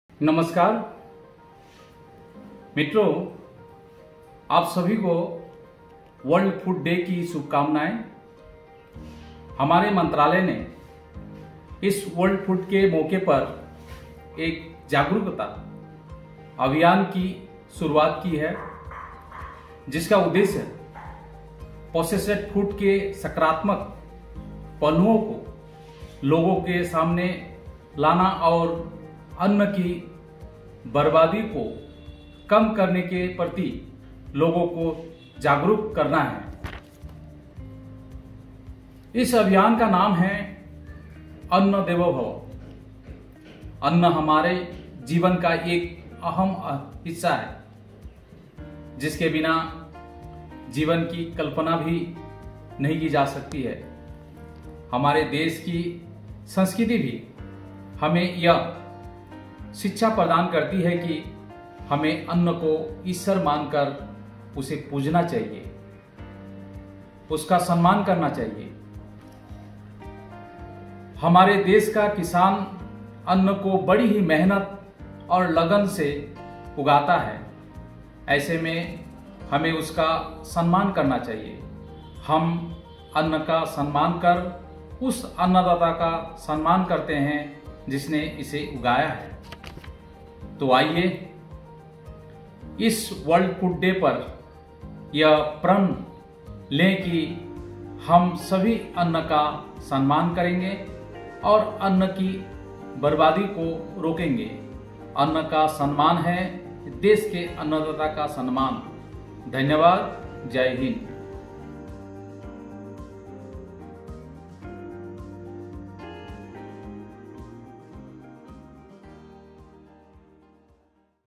हमें अन्न का सम्मान करना चाहिए: श्री रामेश्वर तेली, केंद्रीय राज्य मंत्री
अन्न हमारे जीवन का एक अहम हिस्सा है जिसके बिना जीवन की कल्पना भी नहीं की जा सकती है हमारे देश की संस्कृति भी हमें यह शिक्षा प्रदान करती है कि हमें अन्न को ईश्वर मान कर उसे पूजना चाहिए, उसका सम्मान करना चाहिए: श्री रामेश्वर तेली, केंद्रीय राज्य मंत्री